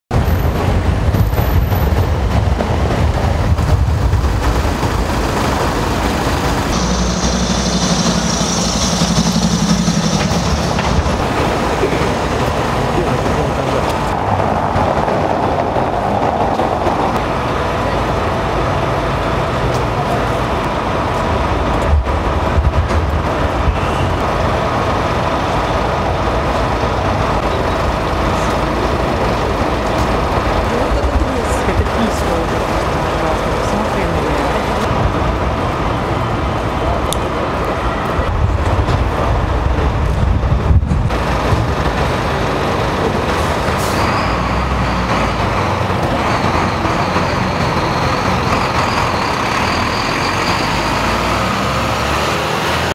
MIX TV: В центре Риги найдена взрывчатка: люди эвакуированы
Смотрите и слушайте радио MixFM Скачивайте приложение Mix Media Radio